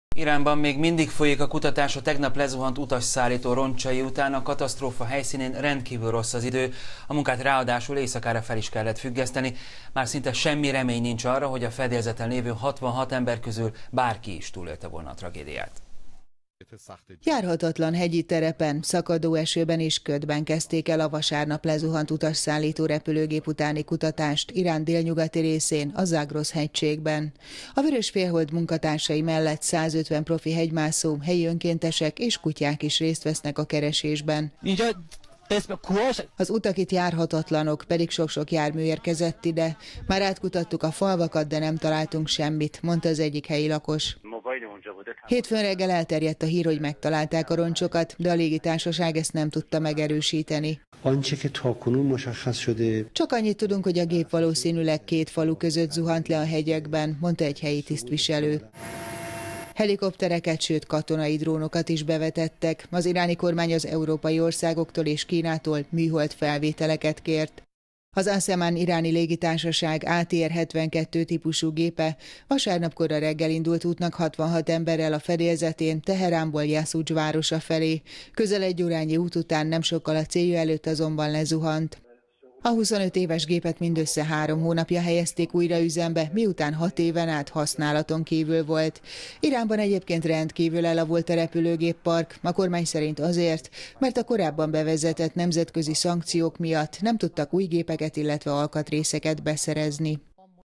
Kutatnak a lezuhant gép roncsai után Iránban
Iránban még mindig folyik a kutatás a tegnap lezuhant utasszállító roncsai után. A katasztrófa helyszínén rendkívül rossz az idő, a munkát ráadásul éjszakára fel kellett függeszteni.